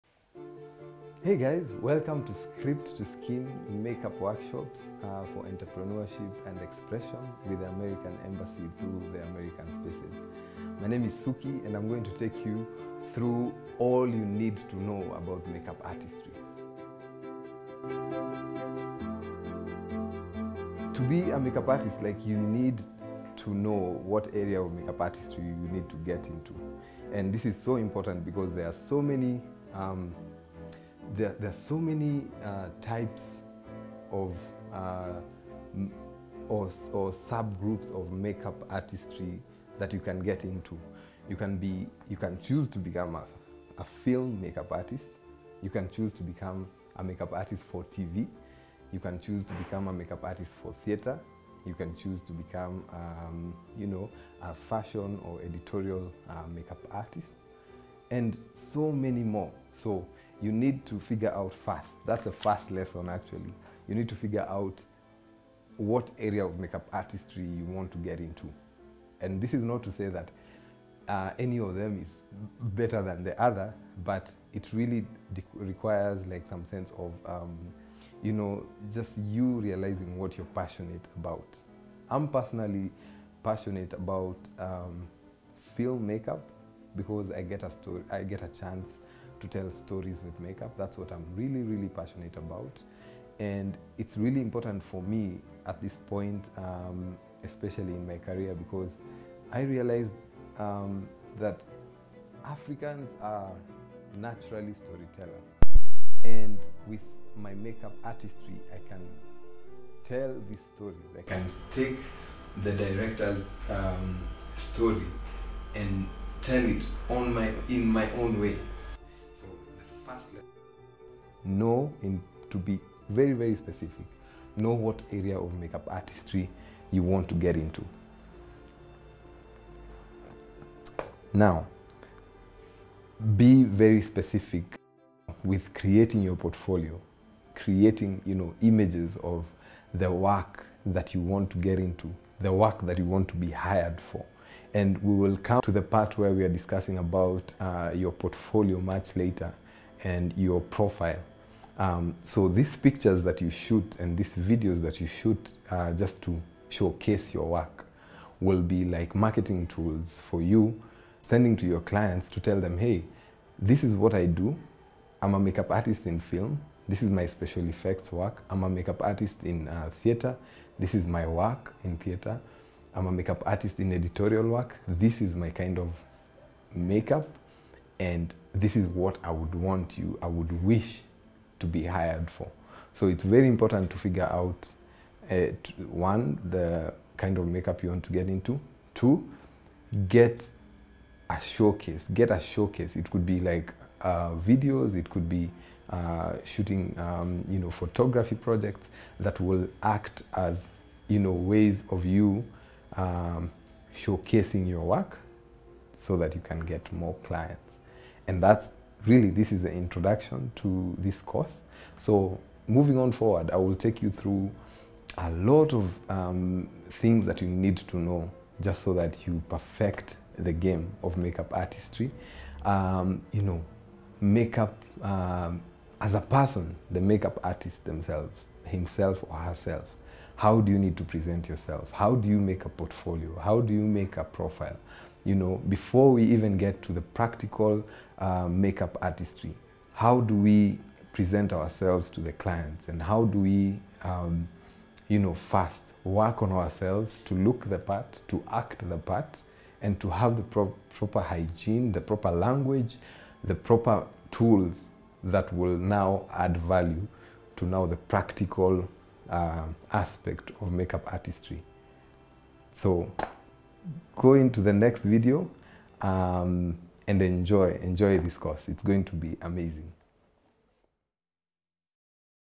SCRIPT TO SKIN LESSON 1 | INTRODUCTION TO MAKEUP ARTISTRY
In order to have a great start to your career you need to figure out what area of makeup artistry you want to venture into.

You could either specialize or combine any of these
-Film makeup
-Special effects makeup
-Bridal makeup
-Tv makeup
-Personalized makeup services
-Fashion/Editorial makeup

While all of these are very viable forms of makeup artistry i do think that choosing what area to venture into has to go inline with your interests & passion so it is an individual decision.The other approach would be trying out most of them to see which one sits well with you.

Don't forget to add your own magic onto whichever area you choose,that is what sets you apart.

Good luck